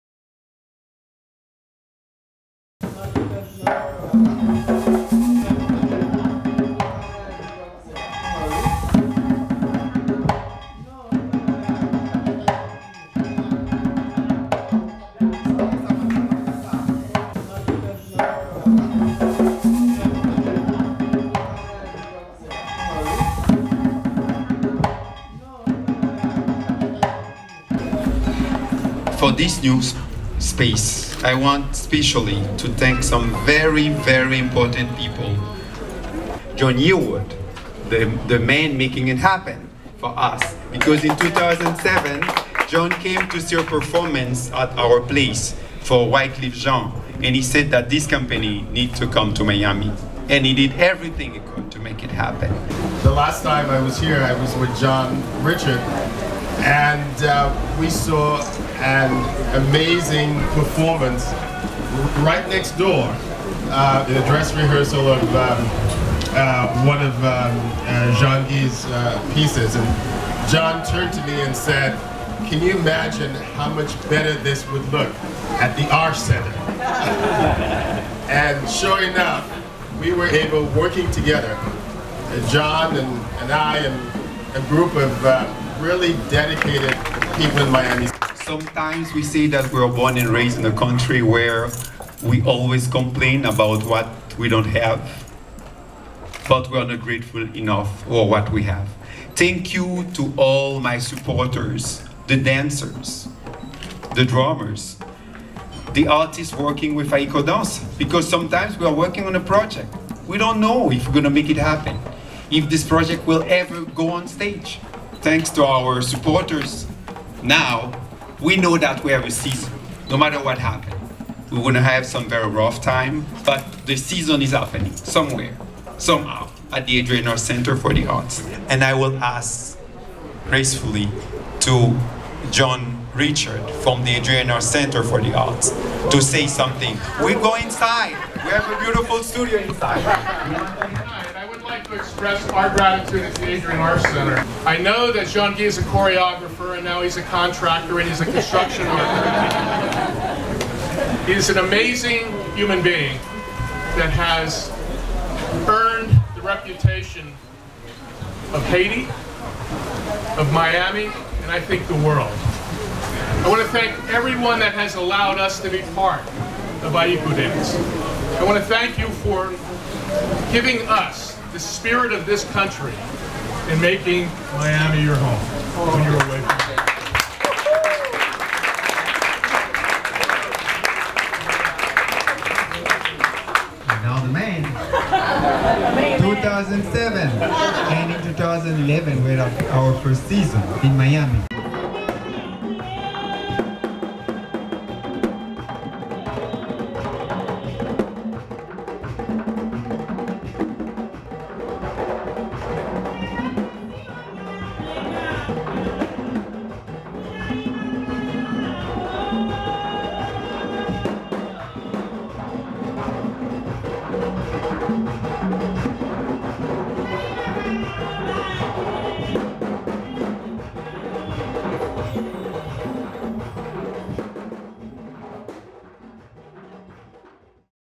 Ayikodans Studio Inauguration Event in Haiti
"[An] intense, thrilling performance!"- The Miami Herald

Following 3 sold-out seasons at the Arsht Center, Ayikodans, "Haiti's premier dance troupe" (Miami New Times), returns to Miami to thrill audiences with a world premiere, Tribulations, and an expanded version of Danse de l'arraignée (Dance of the Spider) -- all performed by the company's incredibly fierce dancers and accompanied by an ensemble of drummers and vocalists.

Created by Artistic Director and Choreographer Jeanguy Saintus, a 2008 Prince Claus Award recipient, Tribulations is a dance in three movements, depicting the familiar temptations that often lead to tribulations -- the things that can either bring us happiness or pain.